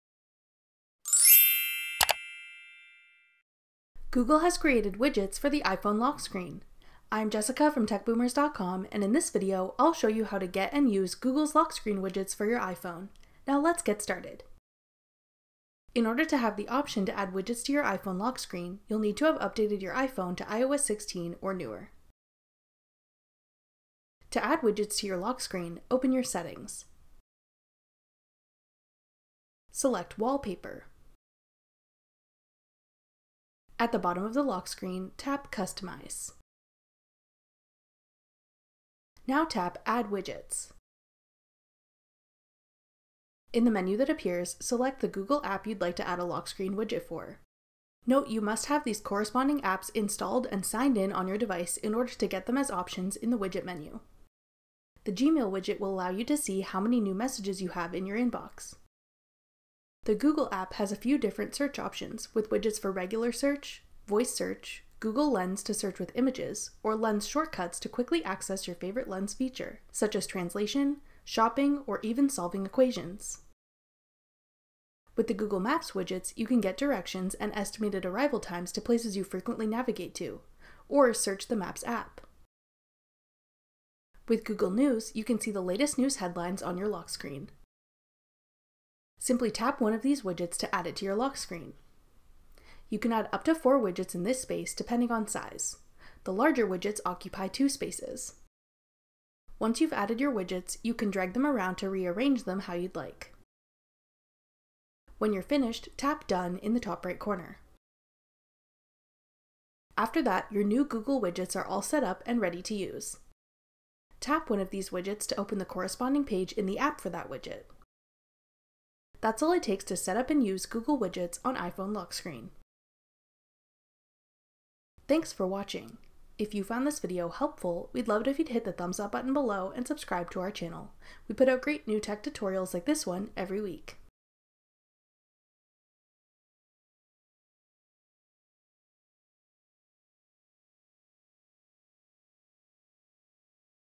How to Get Google Widgets on iPhone Lock Screen
Google has created widgets for the iPhone lock screen! Watch this video to learn how to get widgets for Google, Gmail, Google Maps and Google News on your lock screen on iPhone.

In order to get the option to add widgets to your iPhone lock screen, you will need to have updated your iPhone to iOS16 or newer.

To add widgets to your lock screen, open your Settings. Select Wallpaper. At the bottom of the lock screen, tap Customize. Now tap Add Widgets. In the menu that appears, select the Google app you’d like to add a lock screen widget for. Note you must have these corresponding apps installed and signed in on your device in order to get them as options in the widget menu. The Gmail widget will allow you to see how many new messages you have in your inbox. The Google app has a few different search options, with widgets for regular search, voice search, google lens to search with images, or lens shortcuts to quickly access your favourite lens feature such as translation, shopping, or even solving equations. With the Google Maps widgets, you can get directions and estimated arrival times to places you frequently navigate to, or search the maps app. With Google News, you can see the latest news headlines on your lock screen. Simply tap one of these widgets to add it to your lock screen. You can add up to 4 widgets in this space depending on size. The larger widgets occupy 2 spaces. Once you’ve added your widgets you can drag them around to rearrange them how you’d like. When you’re finished, tap Done in the top right corner. After that, your new Google widgets are all set up and ready to use. Tap one of these widgets to open the corresponding page in the app for that widget.